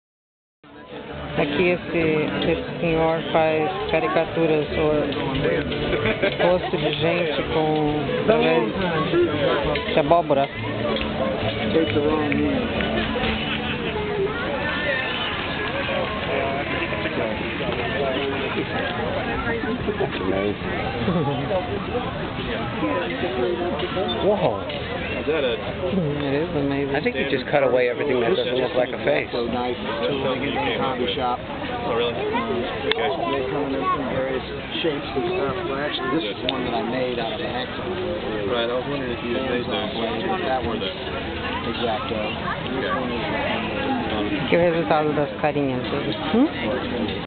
Carving Faces Into Pumpkins
This is an amazing job that this artist does with Pumpkins.. he makes all kinf of faces out of pumpkins..